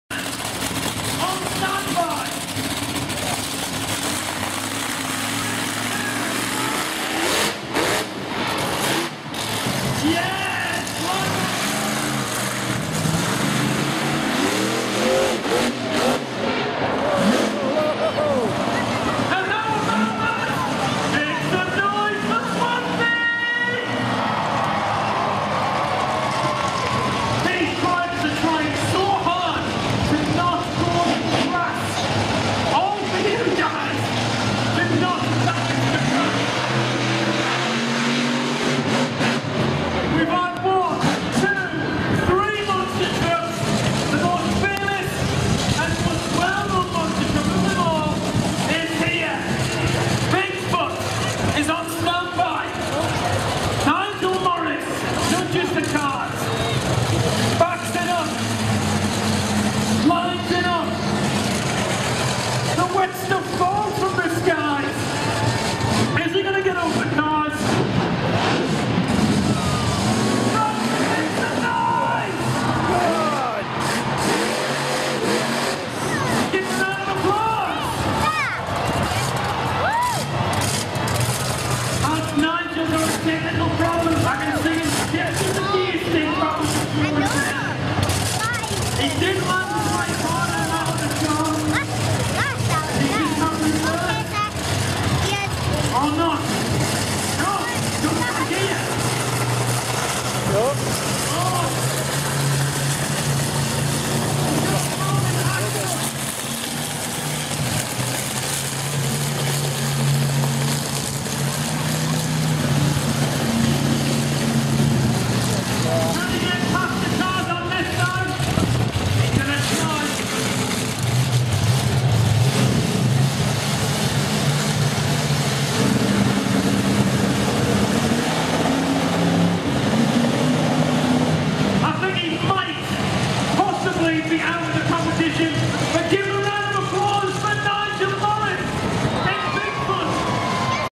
BIGFOOT MONSTER TRUCK | BROKEN AFTER A JUMP
Latest video for your entertainment!

OPEN ME

Footage of Bigfoot breaking something after a small jump at Bradford Bulls Stadium in Bradford

Check out the channels below